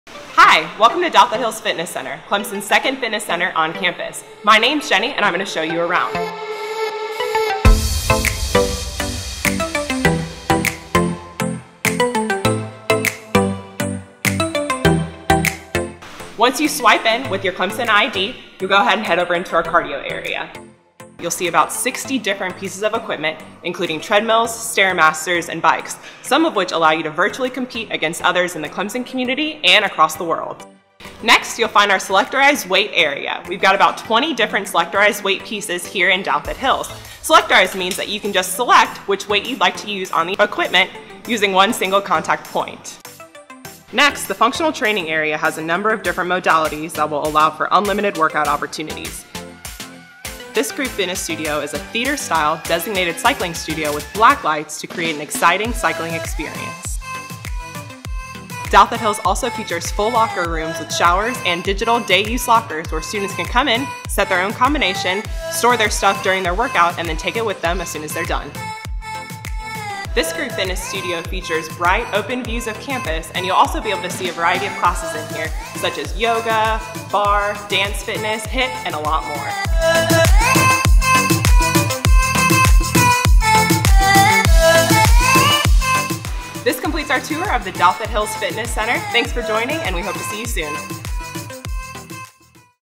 Douthit Hills Fitness Center Tour
Take a look inside Douthit Hills Fitness Center on the second floor of the Douthit Hills Hub building.